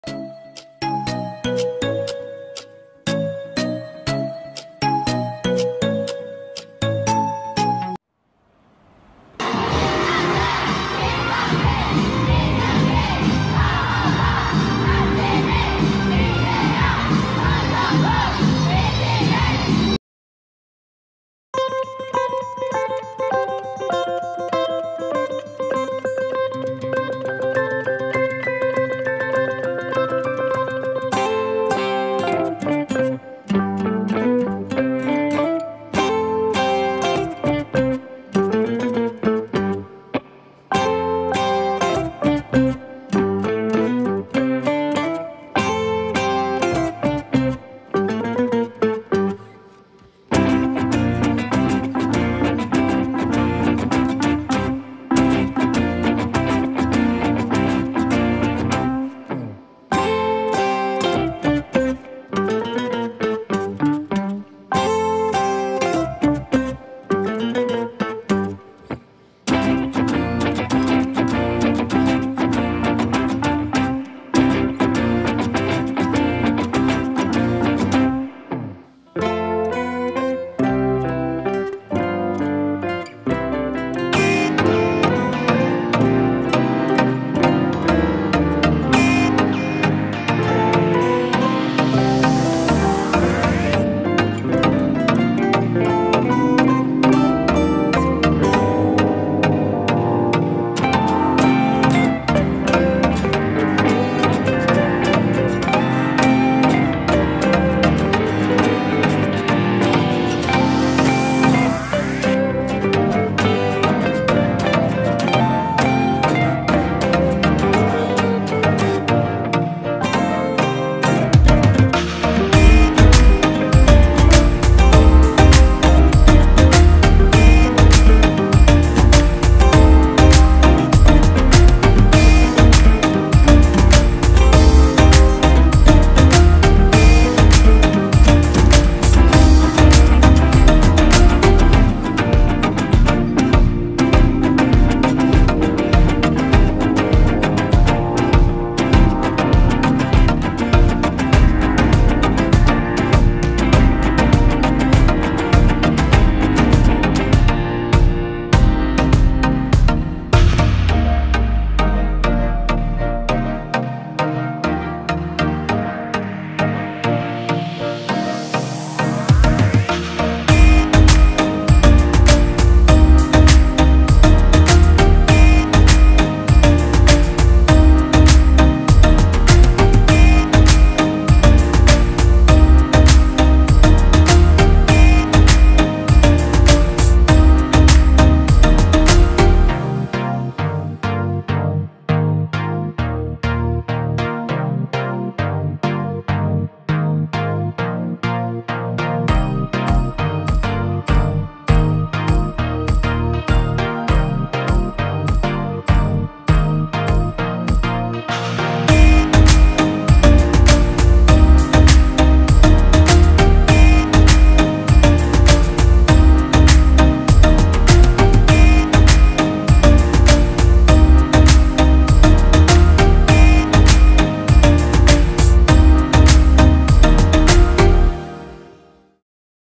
DIY BTS Card (no printer 📠)#2 part-2
Easy BTS DIY card

In this video I’m gonna show you that how to make a BTS DIY card .. I hope you will enjoy this video..